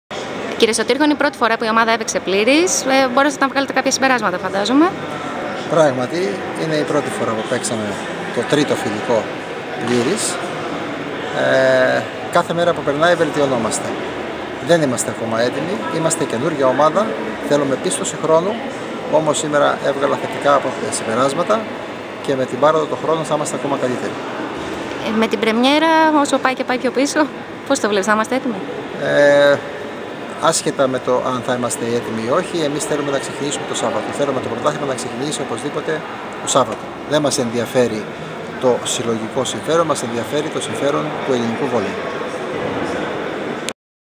ΓΑΥΡΟΣ_ΣΩΤΗΡΧΟΣ
Δηλώσεις του Τέλη Σωτήρχου μετά το φιλικό της ομάδας βόλεϊ Γυναικών του Ολυμπιακού με τον Ηρακλή Κηφισιάς.